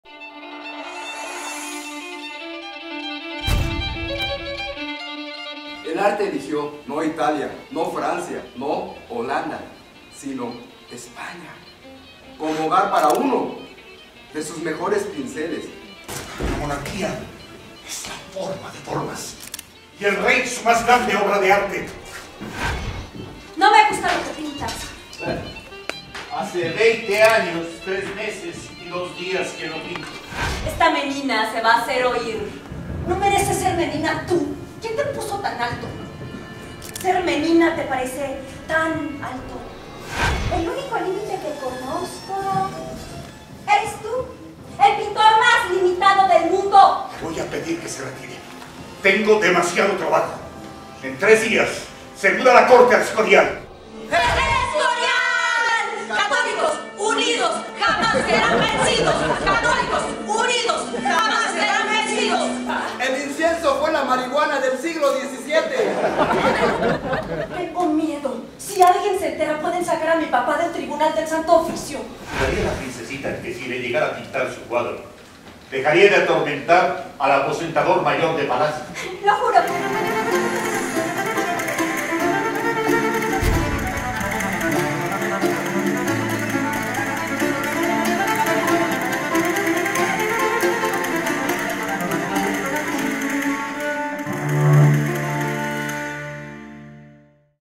LAS MENINAS TEATRO
Obra de teatro de Ernesto Anaya dirigida por Ignacio García, con Aurora Cano, Javier Diaz Dueñas, Violeta Sarmiento, Ichi Balmori, Arturo Vences y Lucas Anaya.